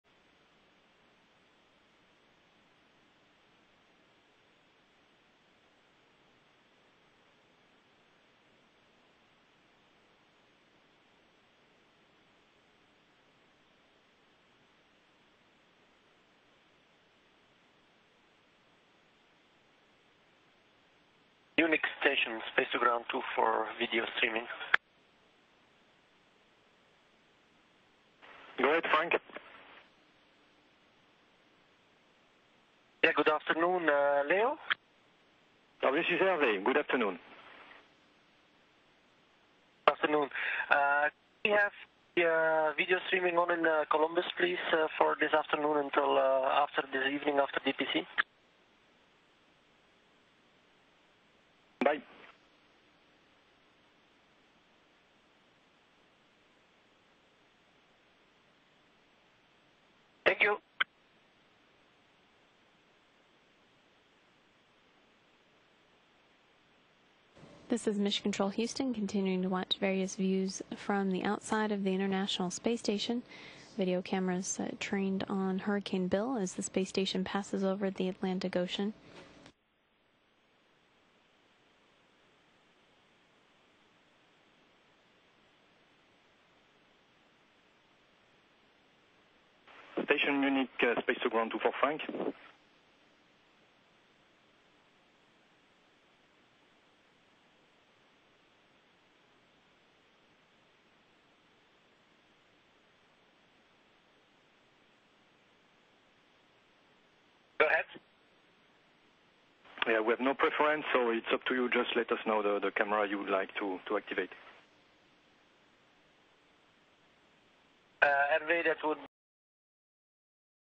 Hurricane Bill
STATION CAMERAS CAPTURE VIEWS OF HURRICANE BILL  KSC (NEW)

From an altitude of 225 statute miles, cameras on the International Space Station
captured spectacular views of Hurricane Bill at 11:46 a.m. EDT on Aug. 17 as the
tropical system churned through the open Atlantic. The National Hurricane Center
forecasts Bill to grow into a major hurricane, but remain isolated off the east
coast of the United States.